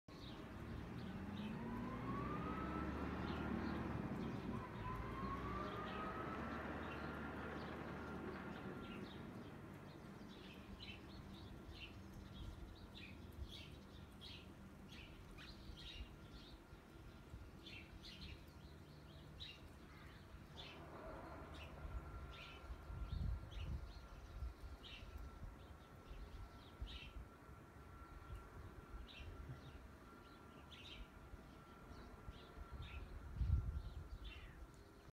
【スイス】レザン(Leysin)で除雪車活動中！【スイス情報.com】
スイスのウインターリゾート、レザン(Leysin)で除雪車が活動中です。

お疲れさま〜

TT